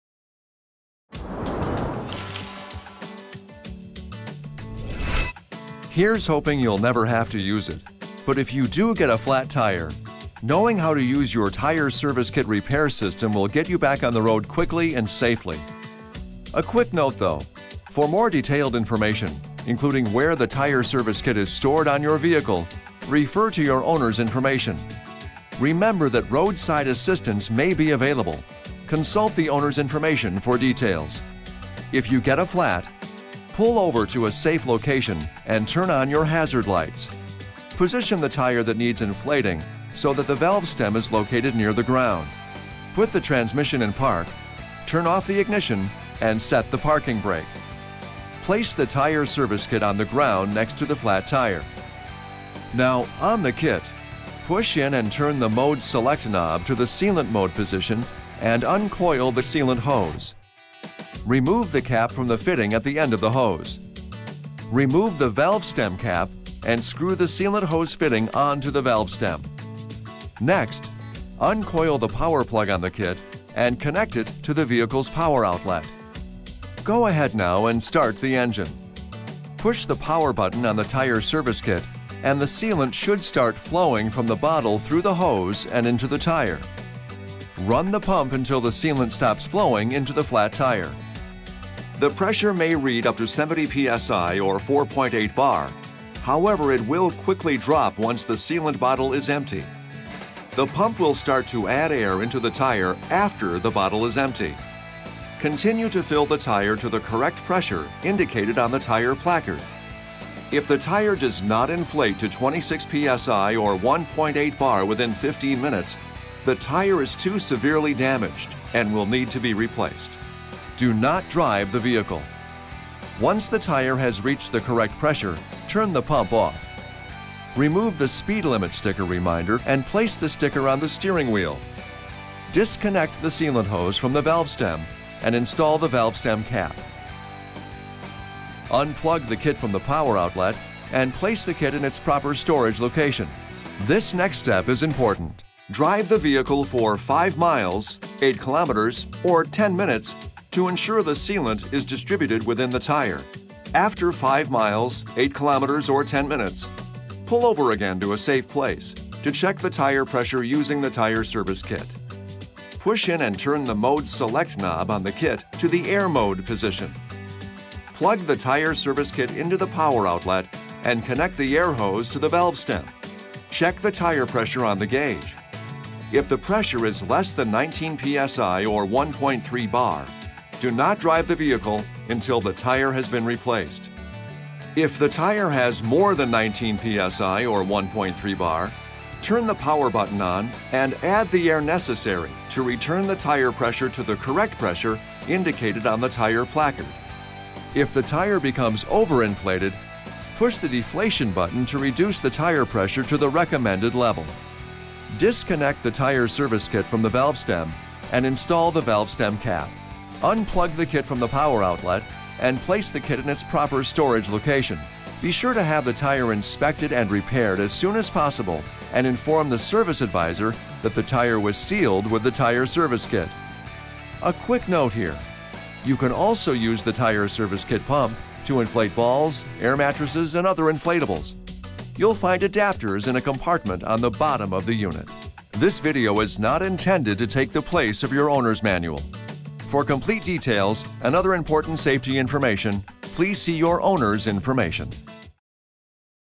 Tire Service Kit-How to fix a flat tire with the tire inflation kit on 2019 Chrysler Pacifica
Learn how to perform emergency tire repair with this handy, on-board accessory.  This video covers how to use the 2019 Chrysler Pacifica flat tire repair kit including: where to position the valve stem when parking, how to connect the filler tube to the tire valve, how to assemble the sealant bottle with the compressor holder, and how to activate this kit in order to inflate the flat tire. You will also learn important information about testing your tires when using this tire inflation kit.